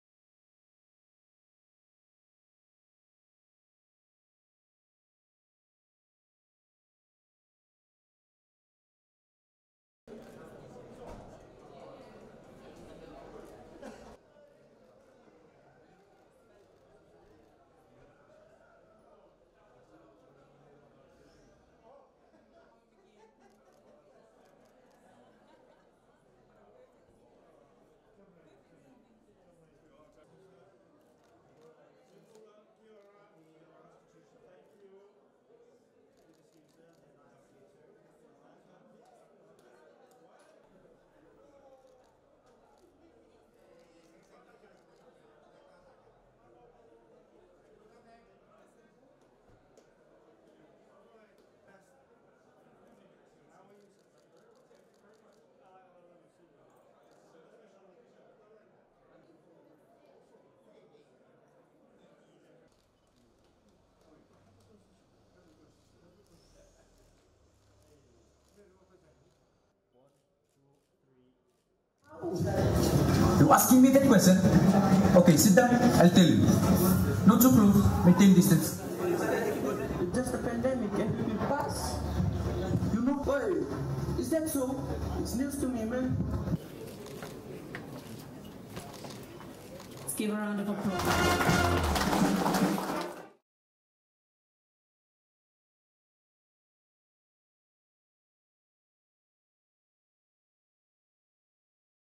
Fijian President held a Constitution Day reception at State House
Fijian President His Excellency Major-General (Ret'd) Jioji Konrote held a  Constitution Day reception at State House.